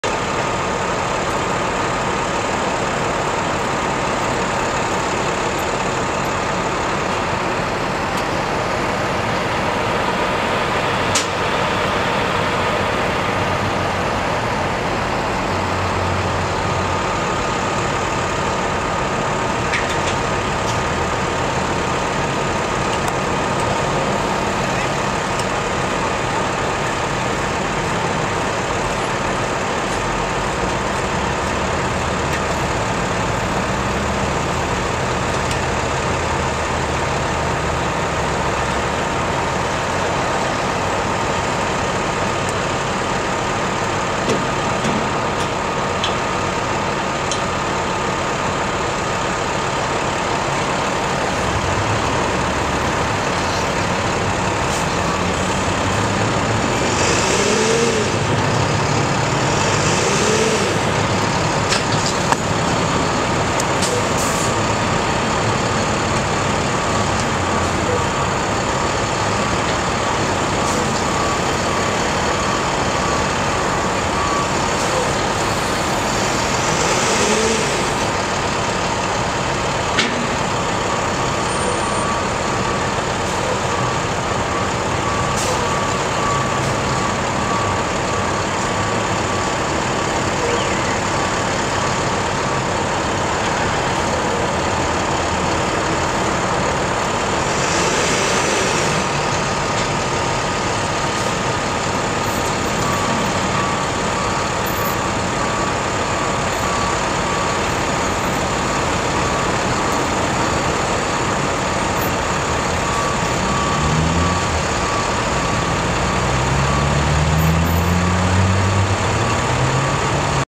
HOLLYWOOD - TOW TRUCKS ON SUNSET BOULEVARD
17 DECEMBRE , HOLLYWOOD , SUR SUNSET BOULEVARD , 3 CAMIONS DE DEPANNAGE DE LA MEME COMPAGNIE , DONT CELUI QUI TRANSPORTE LE CHARIOT ELEVATEUR EST EN PANNE .
L' ACTION ETAIT DE TRANSFERER LE CHARIOT SUR UN AUTRE PORTEUR , ENSUITE DE CHARGER LE PORTEUR EN PANNE SUR L' ENSEMBLE SEMI PORTE - ENGINS , TOUT CECI SUR L' AIRE D' UNE STATION SERVICE SHELL , AVEC RELATIVEMENT PEU DE PLACE .